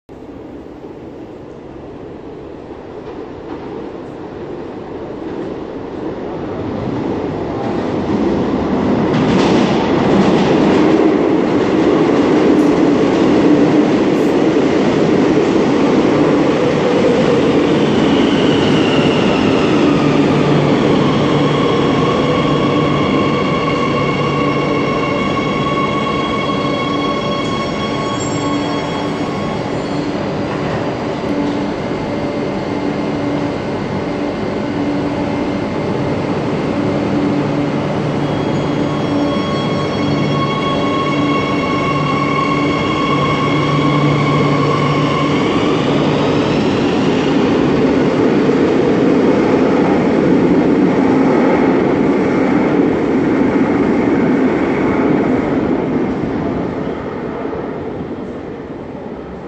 Metro de Madrid L9 - Serie 8400 1ª con faros LED en Barrio del Pilar
♦️♦️♦️ Metro de Madrid ♦️♦️♦️

♦️♦️♦️ Serie: 8400 1ª ♦️♦️♦️

♦️♦️♦️ Línea: L9 ♦️♦️♦️

♦️♦️♦️ Estación: Barrio del Pilar ♦️♦️♦️

♦️♦️♦️ Correspondencia con: Ninguna Correspondencia ♦️♦️♦️